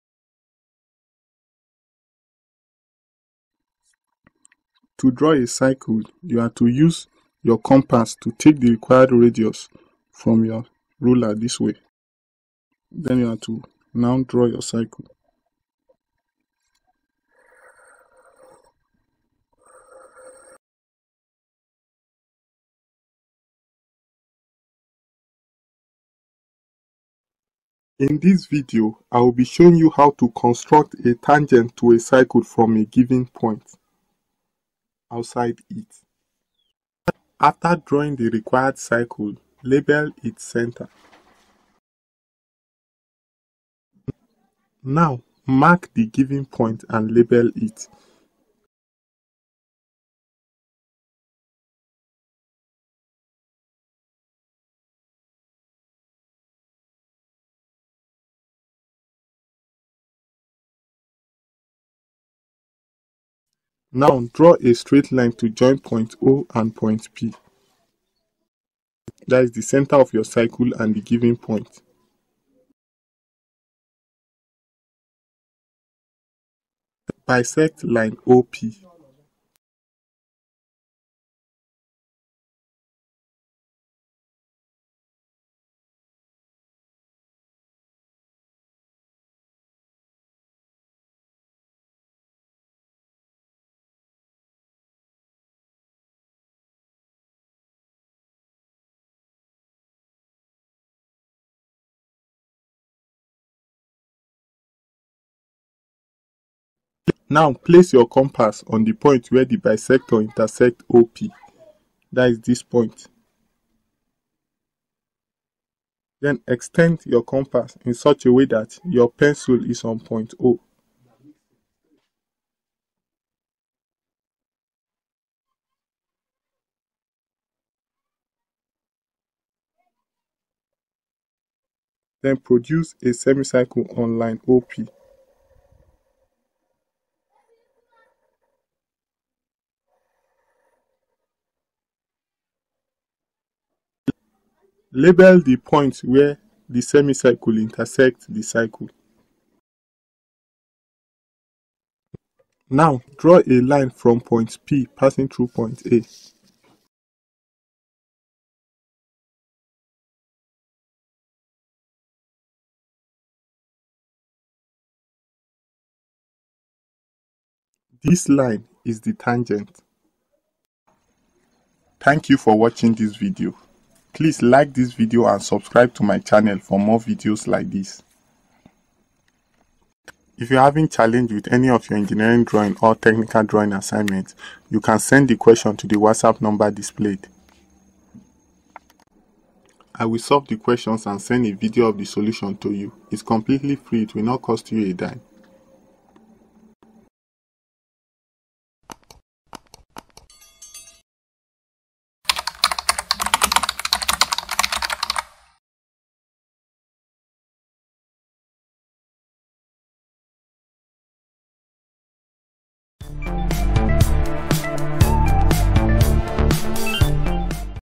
HOW TO CONSTRUCT A TANGENT TO A CIRCLE FROM A GIVEN POINT OUTSIDE IT || Technical drawing
This video explains step by step how to construct a tangent to a circle from a given point outside it.
Instrument Used
Pencil
Ruler
Compass